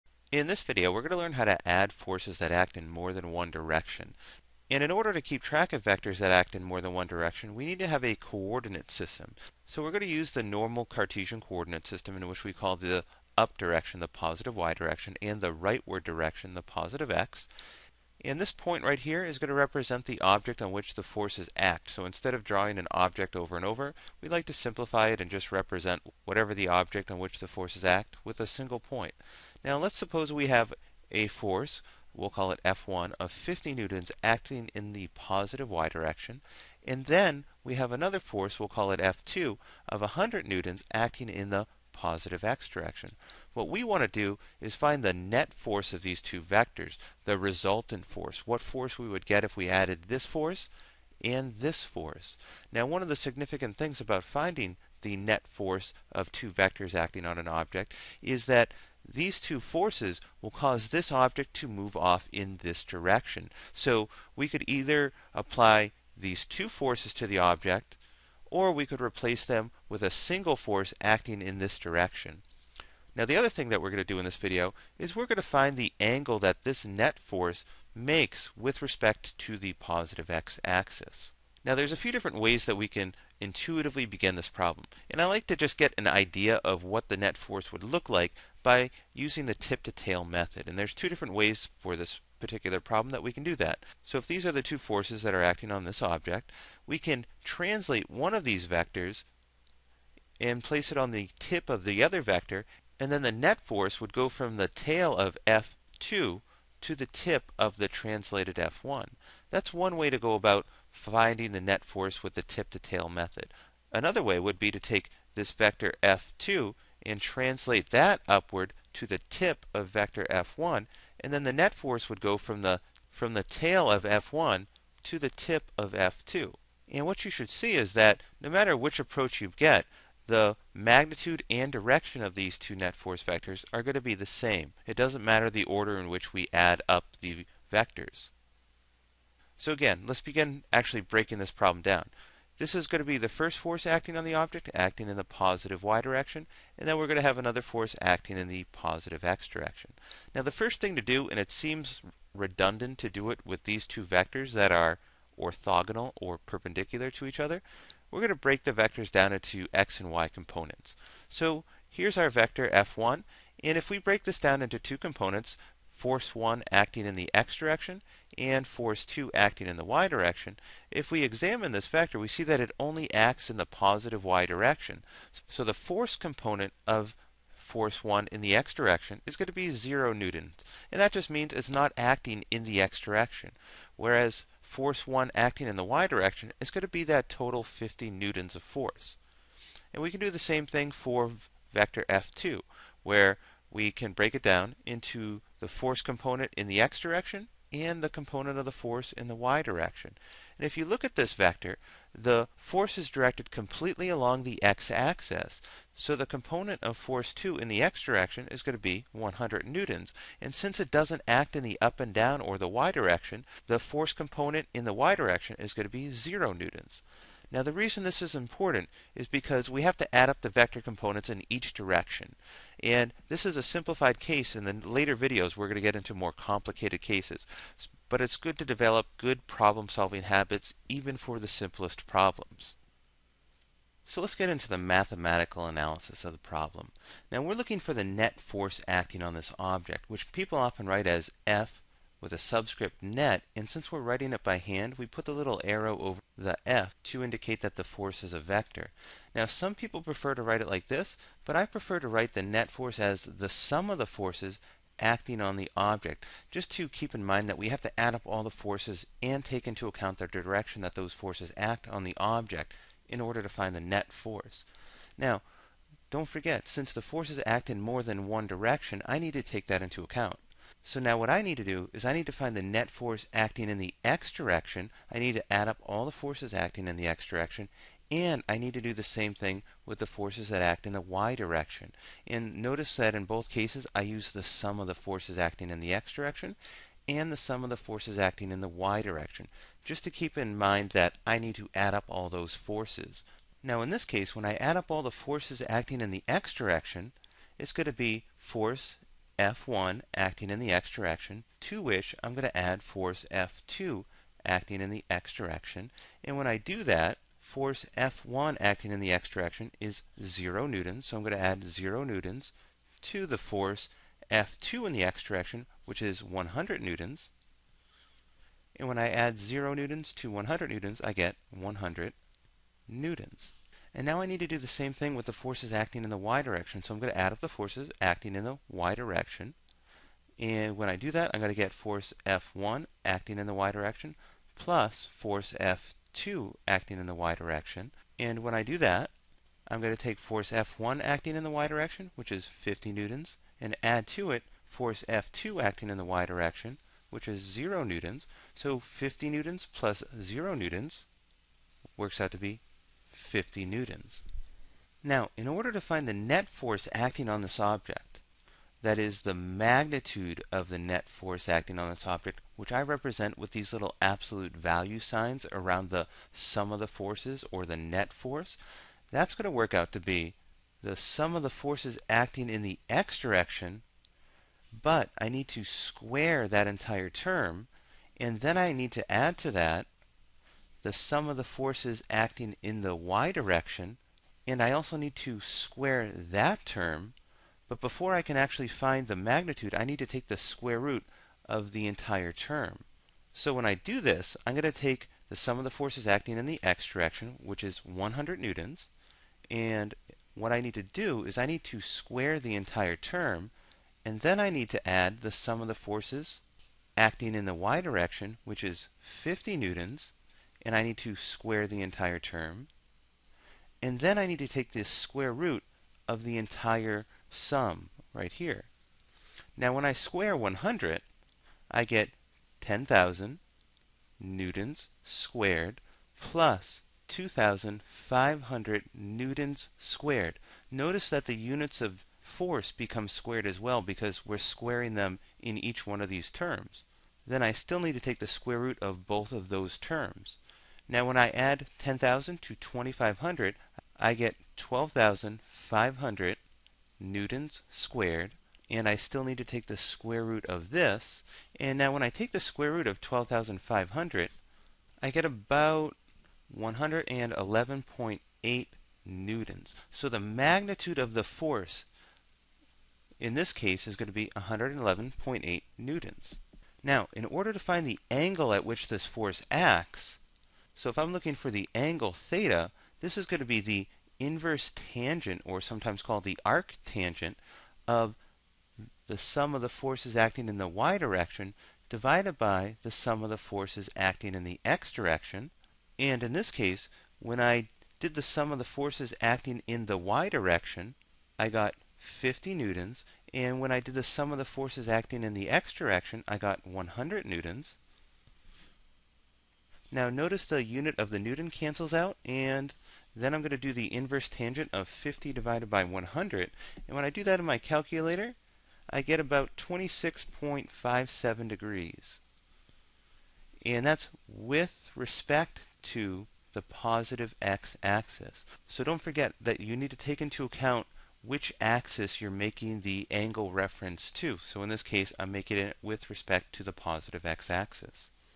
Adding Force Vecotrs in 2 Dimensions Example 1
From PhysicsAccordingtoPalldino.org
Adding two perpendicular force vectors to find the net force acting on an object. Example 1 of many.